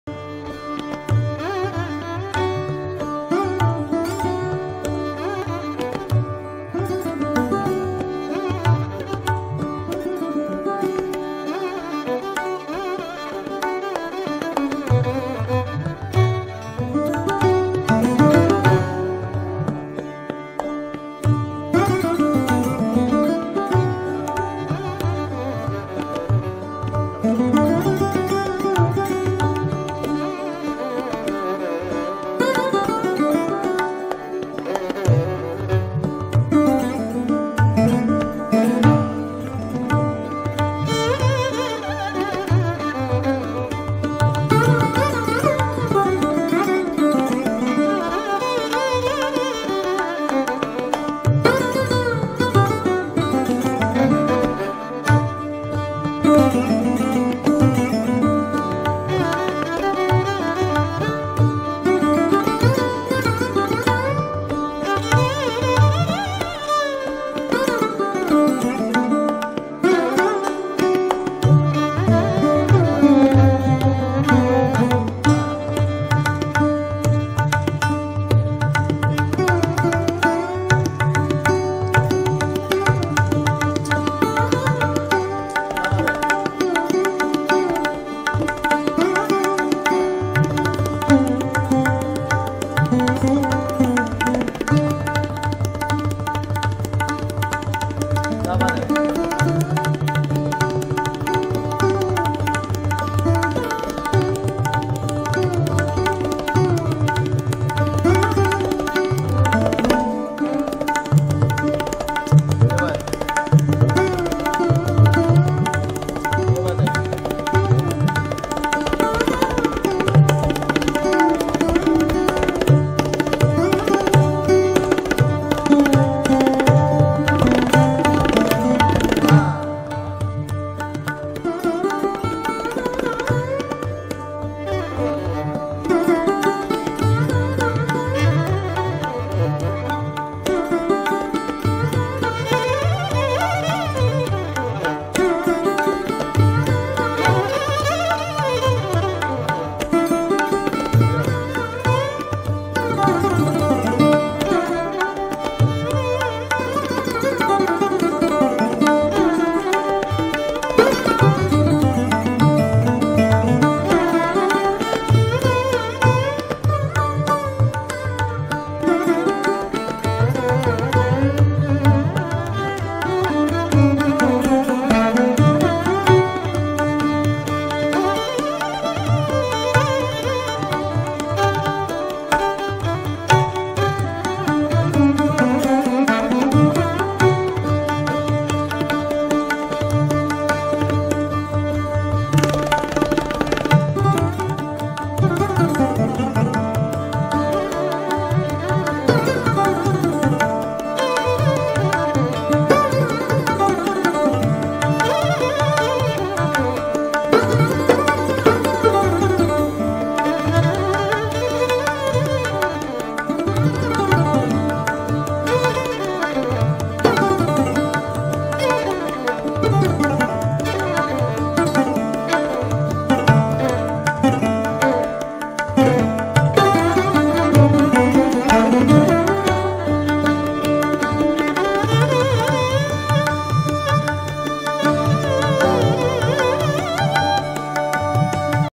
Raag Jhinjoti • Abhishek Borkar - Yadnesh Raikar - Ishaan Ghosh • Sarod - Violin Duet
Raag Jhinjoti • Violin - Sarod Duet with the Young Sarod Maestro Abhishek Borkar along with the dynamic Ishaan Ghosh on Tabla!

~ Riwaayat Festival, IITB Roots 2018